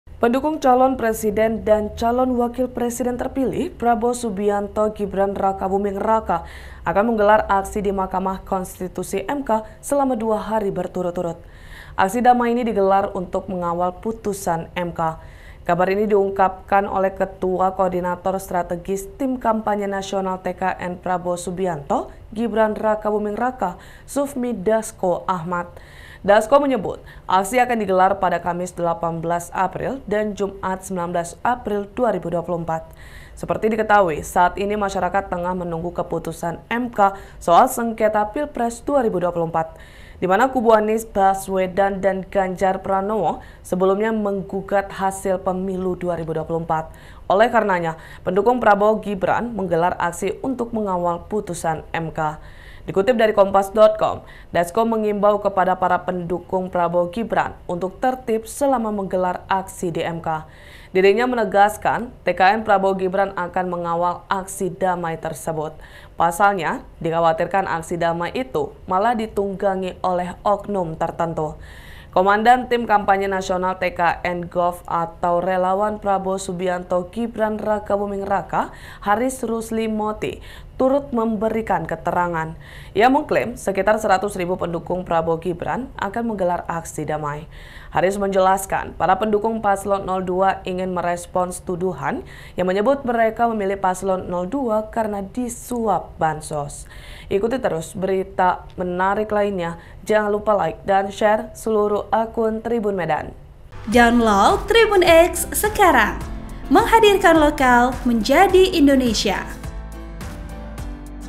GERAM Dituduh Kena Suap Bansos, 100 Ribu Pendukung Prabowo Gelar Aksi Dama Depan MK!
TRIBUN-MEDAN.COM - Pendukung calon presiden dan calon wakil presiden terpilih, Prabowo Subianto-Gibran Rakabuming Raka akan menggelar aksi di Mahkamah Konstitusi (MK) selama dua hari berturut-turut.

Aksi damai ini digelar untuk mengawal putusan MK.

Kabar ini diungkapkan oleh Ketua Koordinator Strategis Tim Kampanye Nasional (TKN) Prabowo Subianto-Gibran Rakabuming Raka, Sufmi Dasco Ahmad.

Dasco menyebut, aksi akan digelar pada Kamis (18/4) dan Jumat (19/4/2024).

Seperti diketahui, saat ini masyarakat tengah menunggu keputusan MK soal sengketa Pilpres 2024.

Di mana kubu Anies Baswedan dan Ganjar Pranowo sebelumnya menggugat hasil Pemilu 2024.

Oleh karenanya, pendukung Prabowo-Gibran menggelar aksi untuk mengawal putusan MK.

Dikutip dari Kompas.com, Dasco mengimbau kepada para pendukung Prabowo-Gibran untuk tertib selama menggelar aksi di MK.

Dirinya menegaskan, TKN Prabowo-Gibran akan mengawal aksi damai tersebut.

Pasalnya, dikhawatirkan aksi damai tersebut malah ditunggangi oleh oknum tertentu.

Komandan Tim Kampanye Nasional (TKN) Golf (Relawan) Prabowo Subianto-Gibran Rakabuming Raka, Haris Rusly Moti turut memberikan keterangan.

Ia mengklaim, sekitar 100 ribu pendukung Prabowo-Gibran akan menggelar aksi damai.

Haris menjelaskan, para pendukung paslon 02 ingin merespons tuduhan yang menyebut mereka memilih paslon 02 karena disuap bansos.